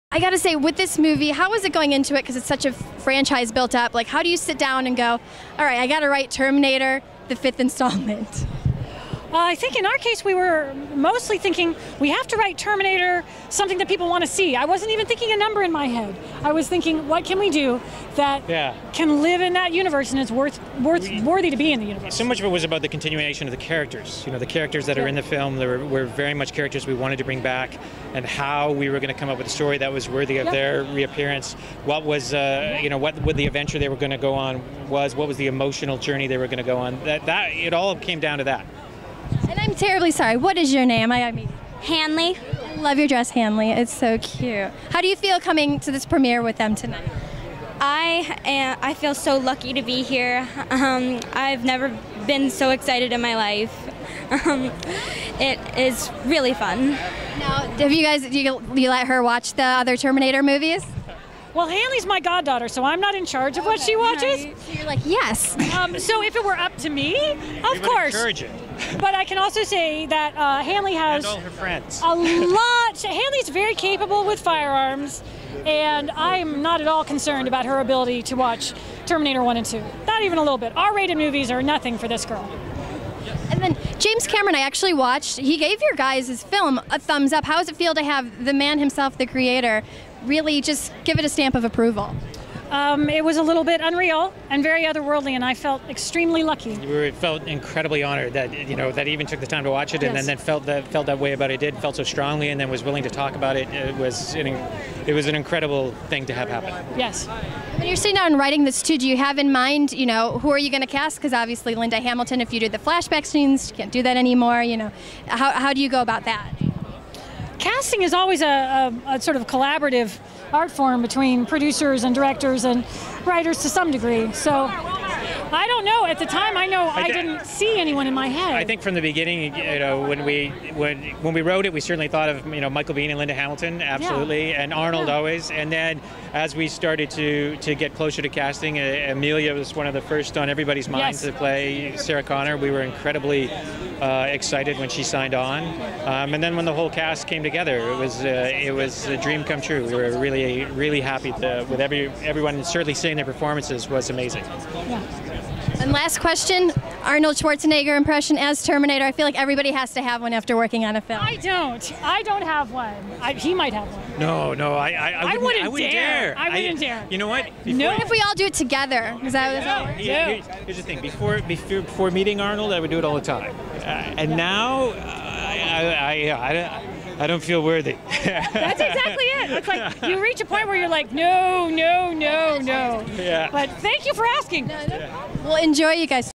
Laeta Kalogridis and Patrick Lussier at the Terminator Genisys Premiere
Host Kristin Carole chats with writers Laeta Kalogridis and Patrick Lussier at the Terminator Genisys premiere!

for all your movie news!

Shopping on Amazon?